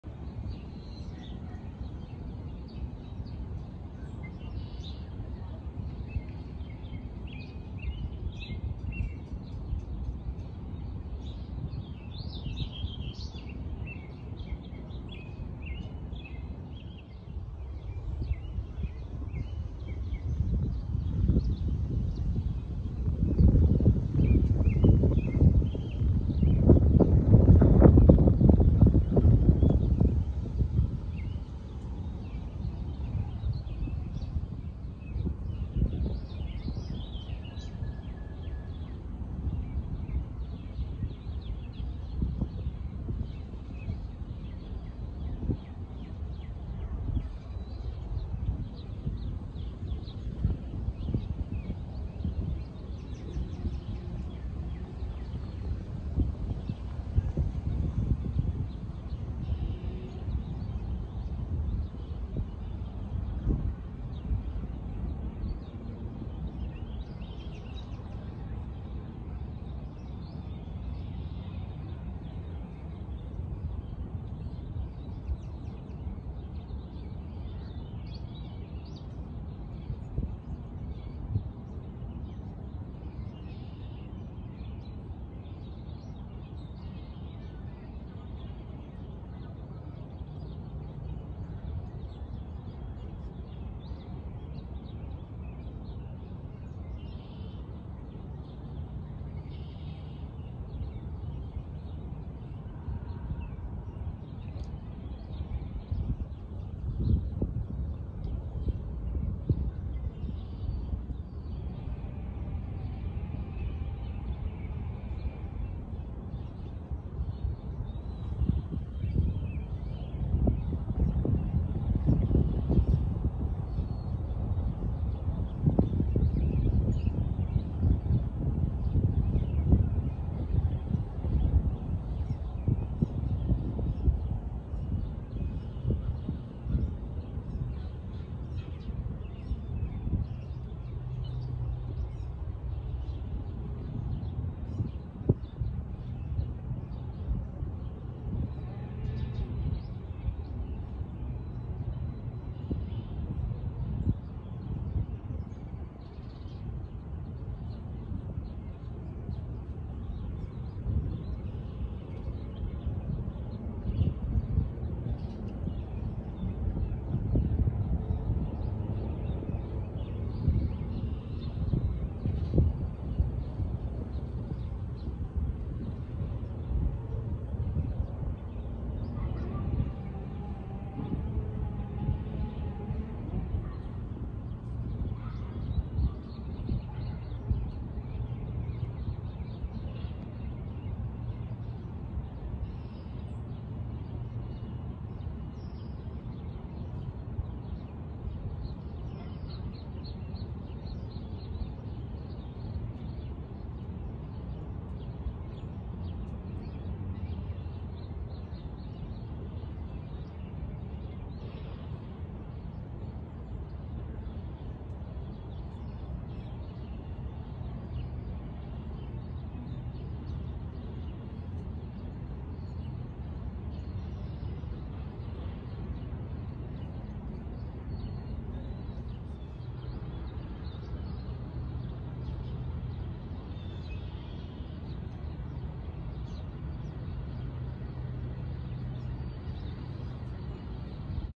The Grenchus Foundation ~ 4 minute Meditation Video - Sounds of Nature
Mini Meditation Video:

Breathe in. Breathe out. again, inhale. exhale, listen to the nature that surrounds you. allow yourself to be carried away by the sounds of nature. let the wind remove anything negative you have been holding on to. begin to center into your heart ask yourself what you are most grateful for today. inhale. exhale. inhale...exhale. look more deeply inside of yourself and see .how is God calling you to a deeper state of peace today?. inhale...exhale...let go of anything negative you are holding on to. release it and let go... inhale... exhale...begin to come out of your meditation and be at one and at peace with yourself and others
The Grenchus Foundation